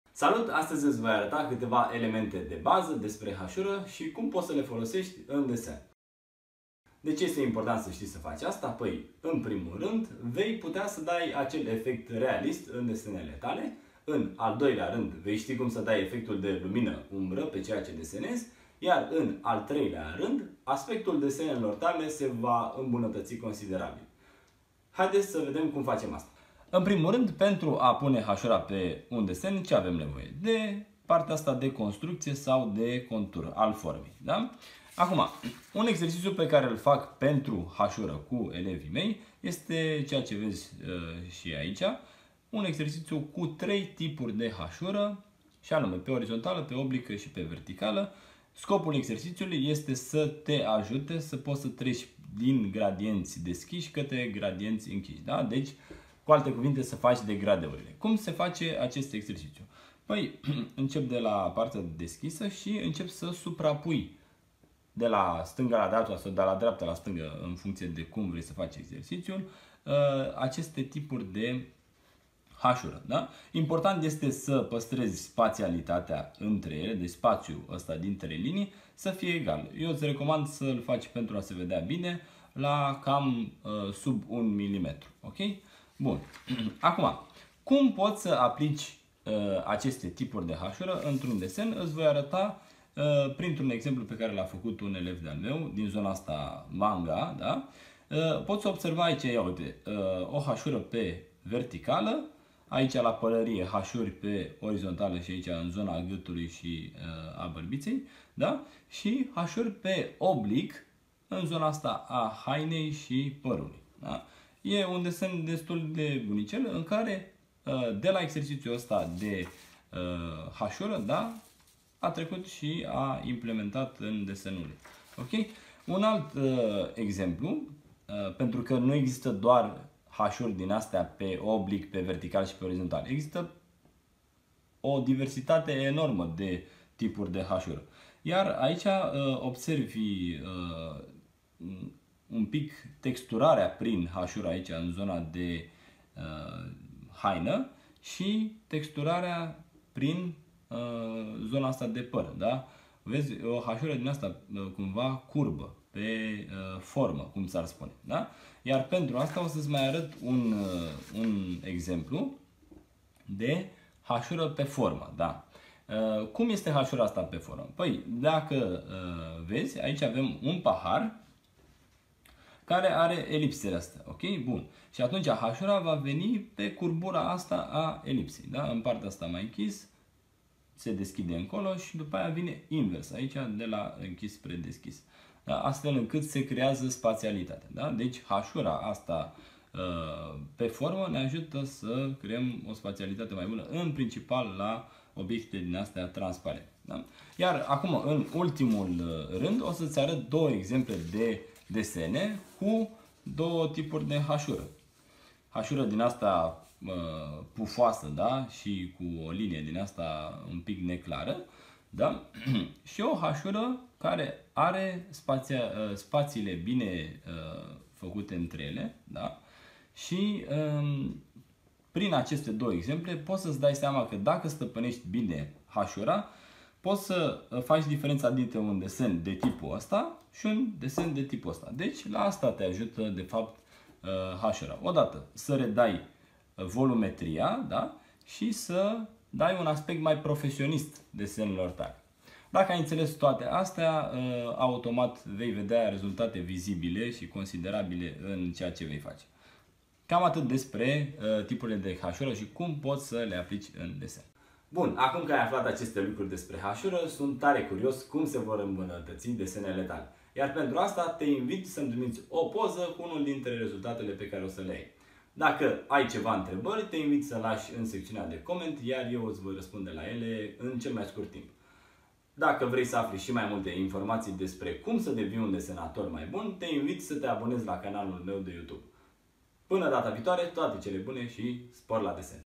Cum fac hasura in desen cu Andrei Radu
In acest tutorial vei afla cateva elemente de baza despre hasura.

Tot aici vei vedea si un exercitiu util pentru a iti exersa atat calitatea hasurii cat si crearea degradeurilor.

De asemenea vei vedea si diferenta dintre un desen cu hasura de incepator si  hasura de avansat.

Daca ai intrebari legate de aceasta tema, poti lasa un comentariu sau imi poti trimite un e-mail la si iti voi raspunde cat mai repede

Daca esti interesat sa iti imbunatatesti tehnica de lucru sau vrei sa te apuci de desen ma poti contacta fie pe e-mailul de mai sus fie la telefon: .

Toate cele bune si spor la desen!